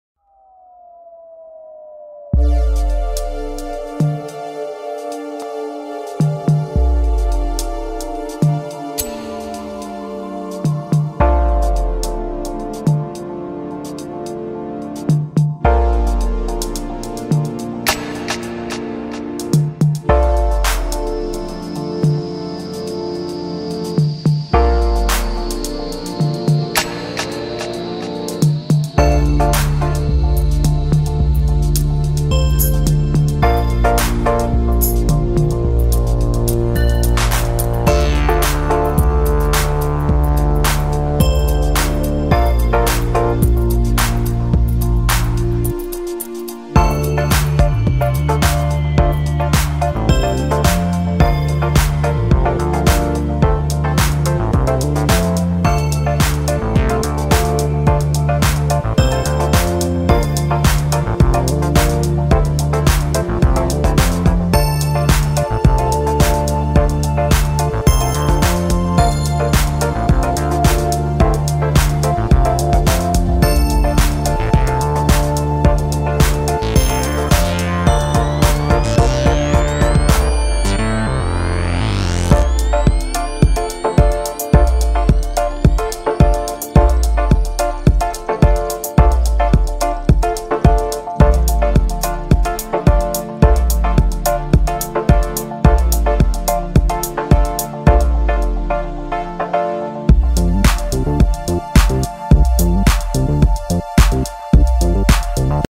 Meeru Island Resort & Spa lagoon cleaning
Among many activities, Meeru did celebrate WTD20, lagoon cleaning was one of them.
The 2020 World Tourism Day theme is about promoting culture, natural heritage and safeguarding endangered species: all aspects resonating with Meeru. The Maldives archipelago consists of 99% ocean. We care about our underwater resources, strive hard to regenerate our coral reef and keep our lagoon clean for all of our biodiversity.